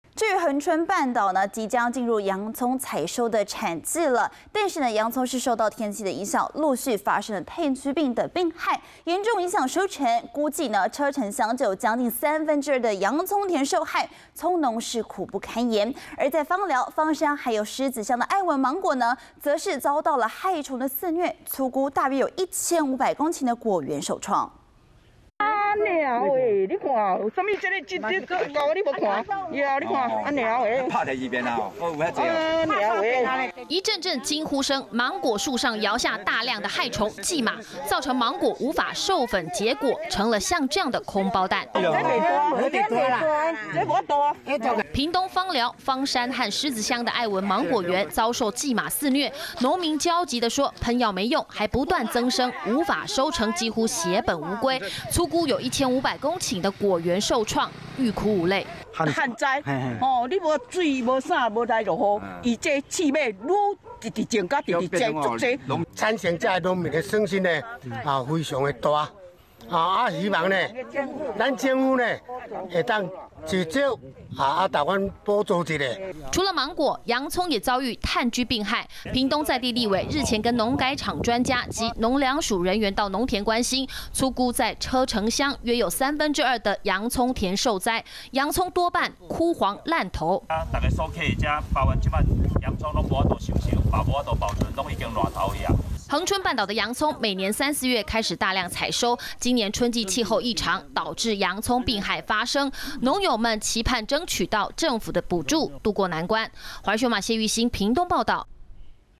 洋蔥.芒果害蟲肆虐 農民喊話政府救助｜華視新聞 20210328
屏東恆春半島，將要進入洋蔥採收產季，洋蔥田受到氣候異常的影響，陸續發生炭疽病等病害，嚴重影響收成，估計車城鄉就有近2/3的洋蔥田受害，蔥農苦不堪言。而在枋寮，枋山和獅子鄉的愛文芒果，則是遭到害蟲「薊馬」肆虐，粗估約有1500公頃果園受創，芒果無法授粉結果，果園恐怕收成無望，期盼政府能盡快救助。